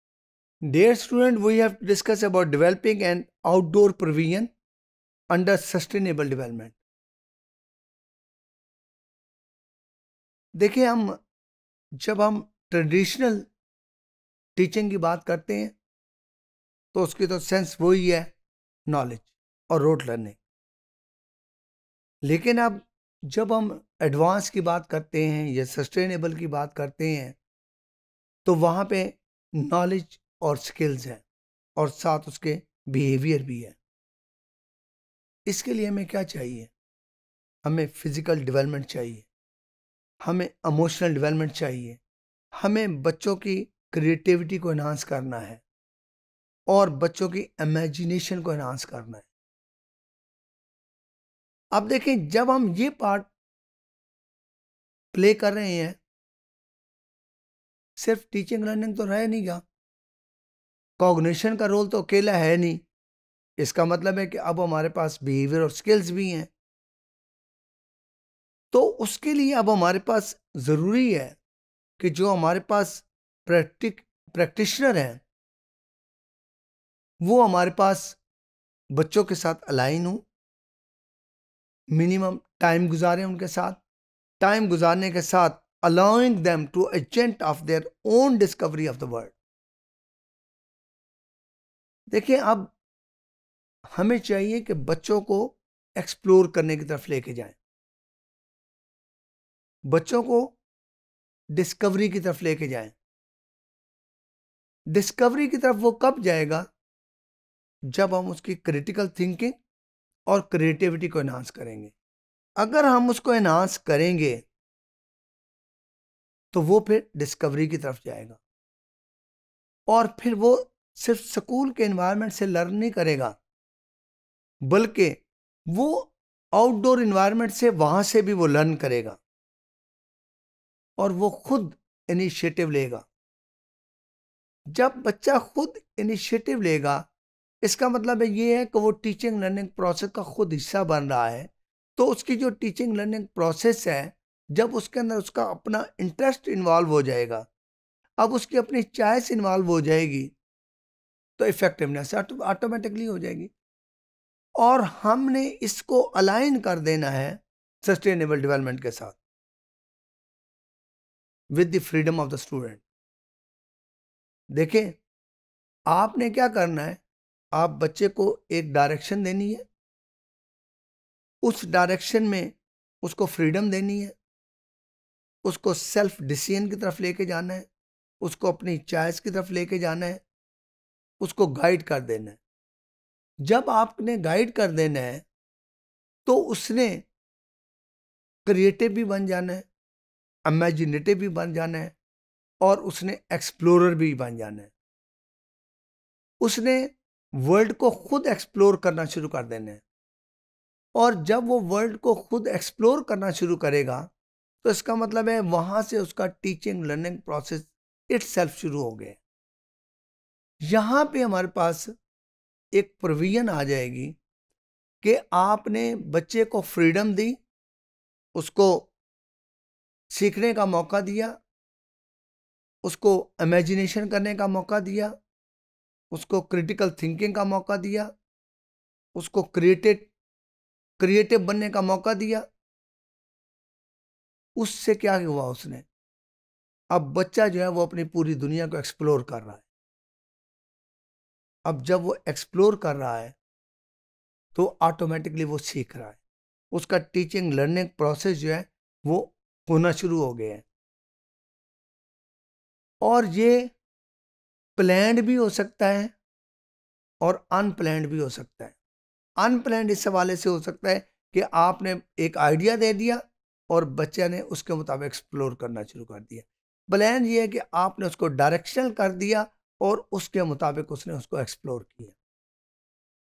Outdoor | Education for Sustainable Development in Early Childhood Education | ECE302_Topic242
Topic242 - Developing an Outdoor Provision Under Sustainable Development,
By Dr. Aamir Hashmi